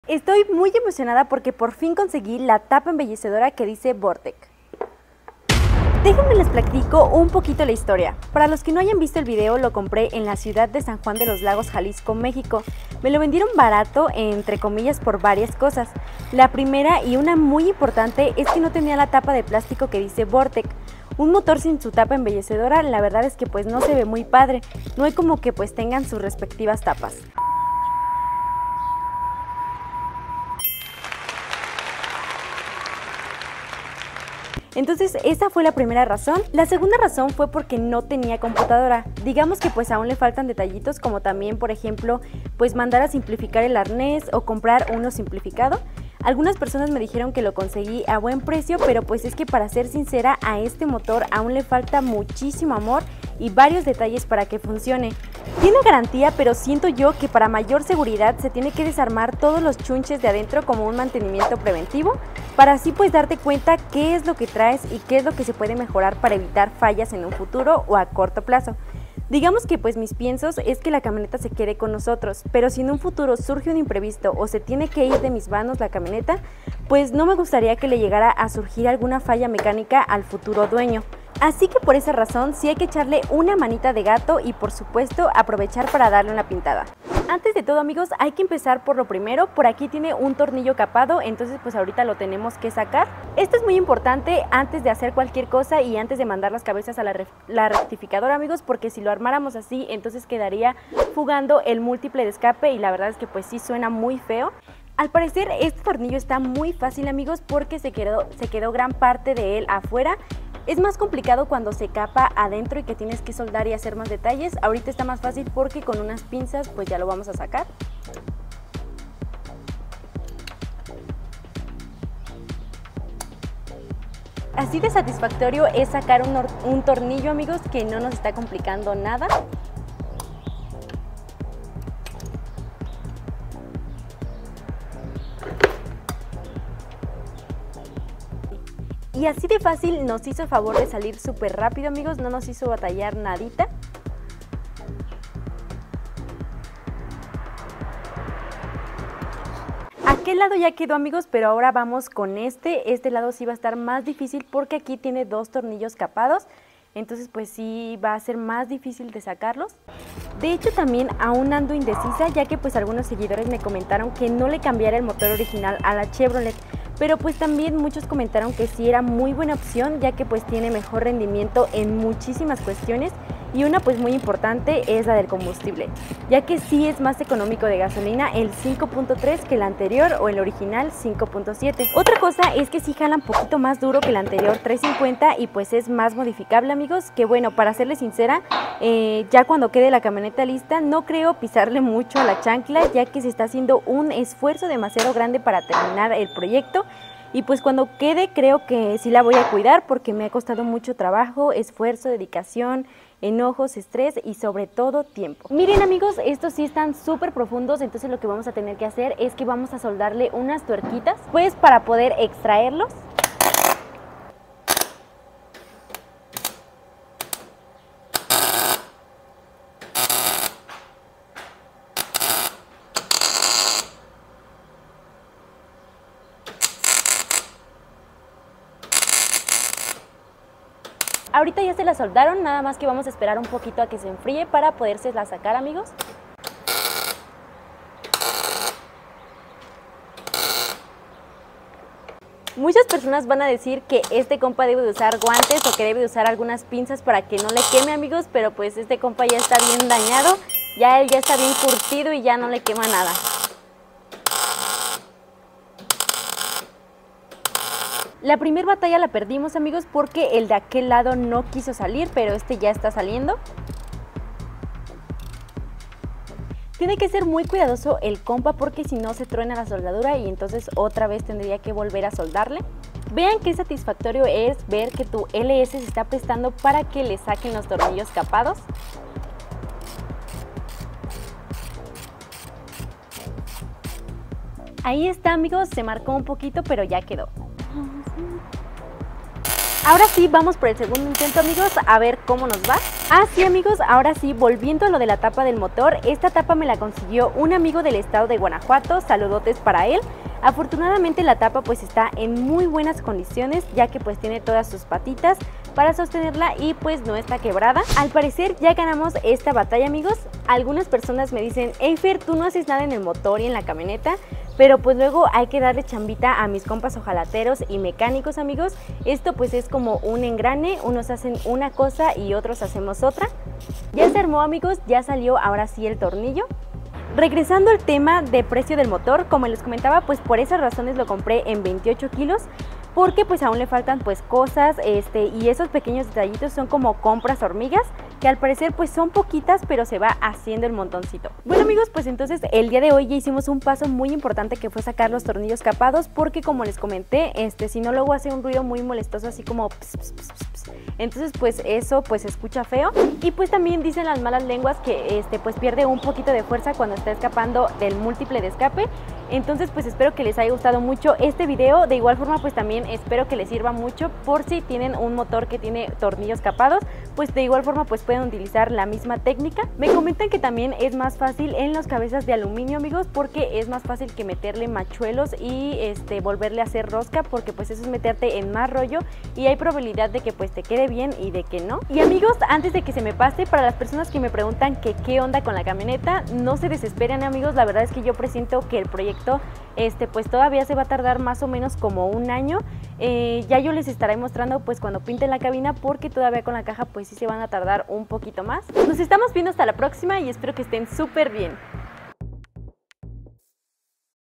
Compré la tapa de MOTOR VORTEC para el swap Ls 🔩comenzamos a repararlo salieron Tornillos capados
Qué tal amigos por aqui les dejo este video espero les guste mucho 🚘